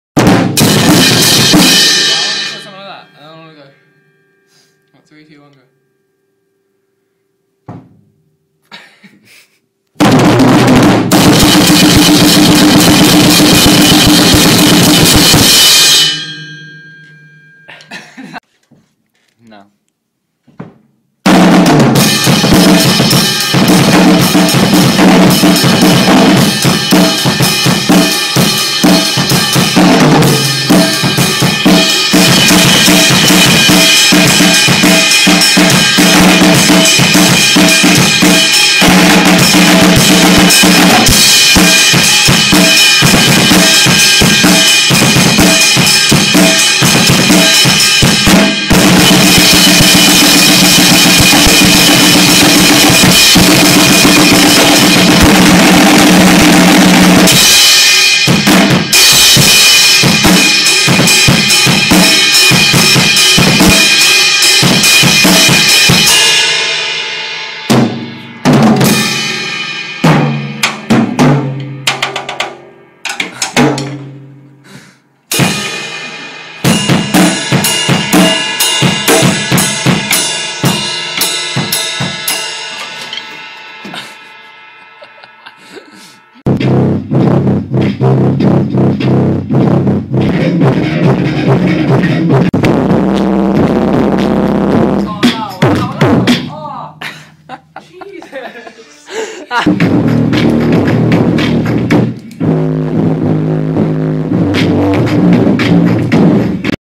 Drums, Synth, Hyper, RAVE!
Drums, Synth, Hyper, RAVE! WAHOO. So bored. Made a video of RANDOM out of time drumming. :L